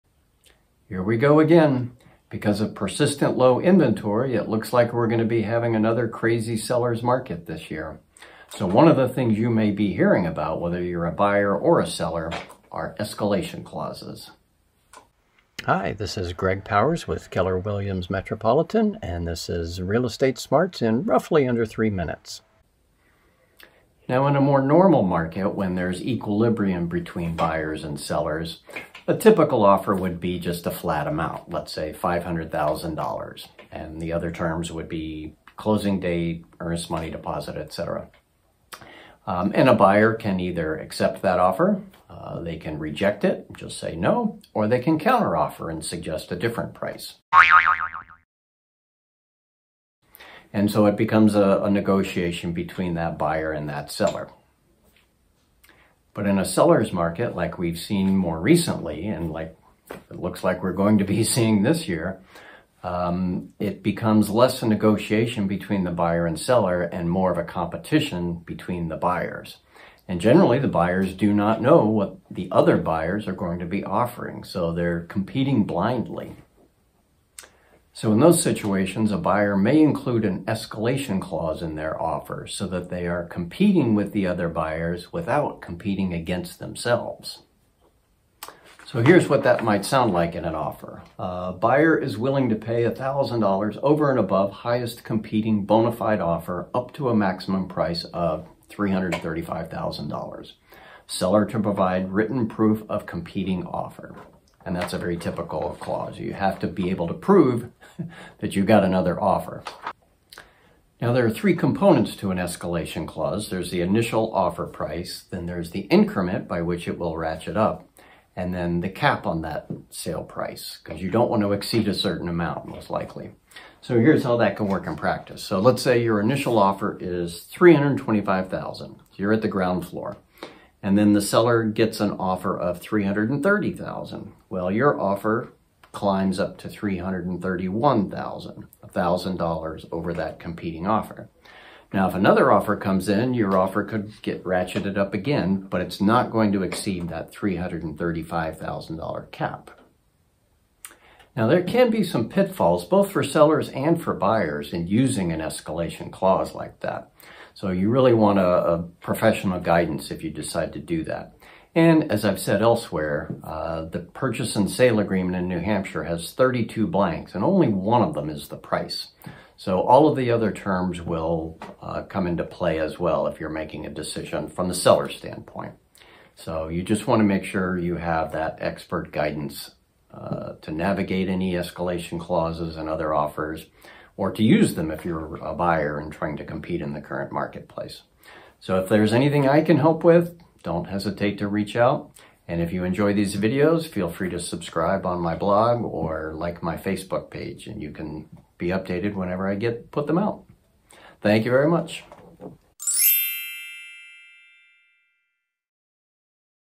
Escalation Clause? What Is That?
In a real estate market that favors sellers, buyers may resort to using an Escalation Clause in their offer to compete with other buyers without competing with themselves.